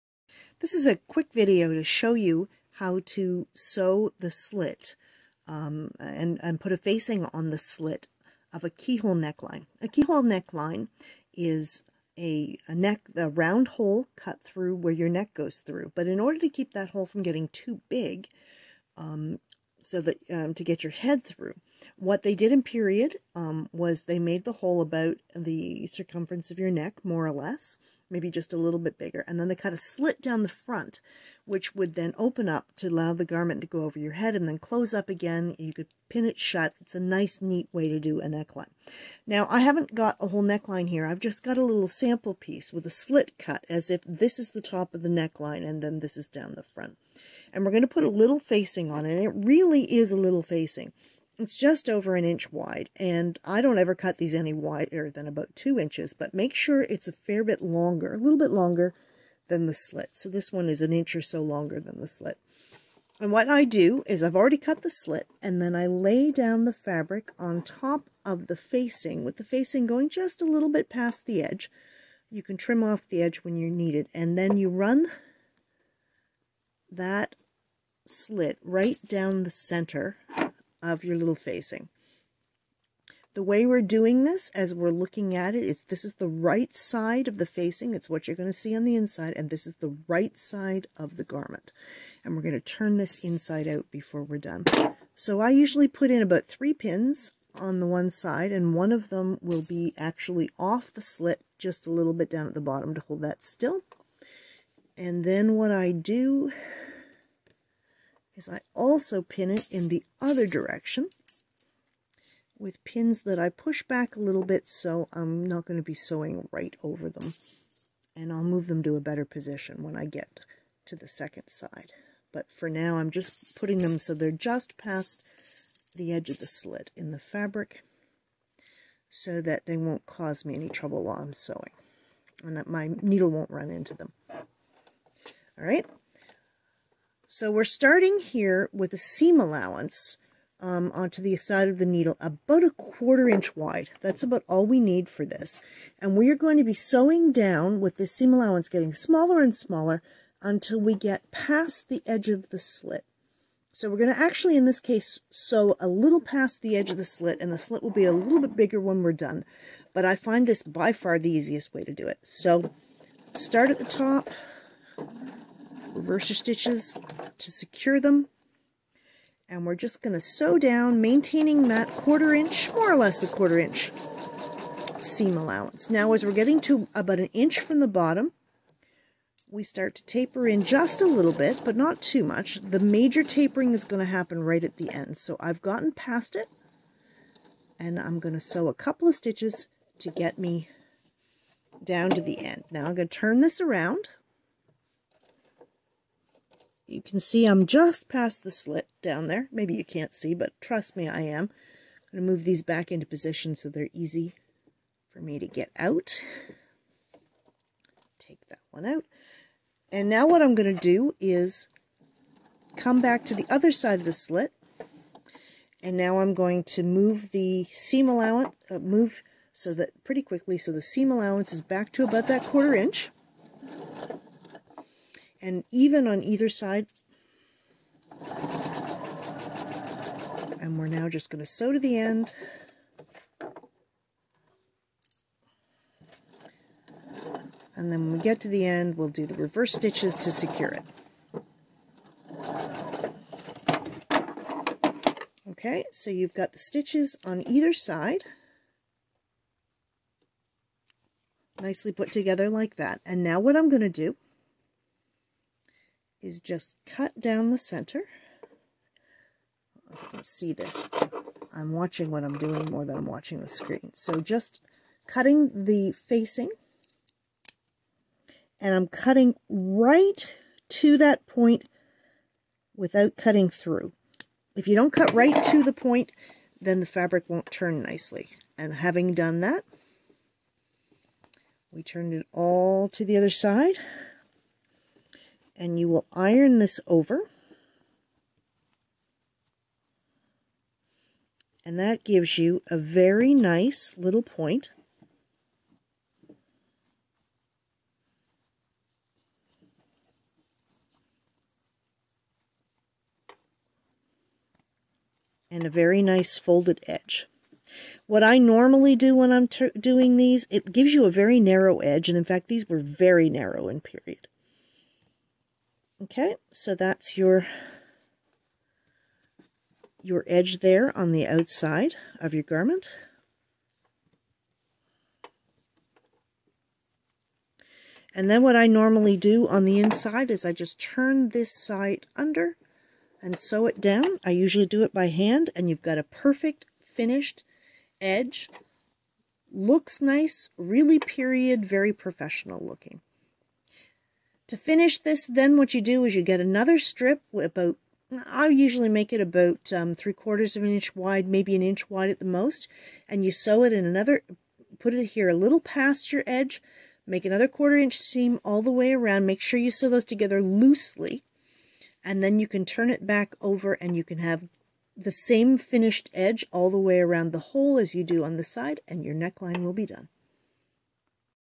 T-Tunic: Sewing the neck slit
Dame Sarra Graeham shows how to sew a tunic neckline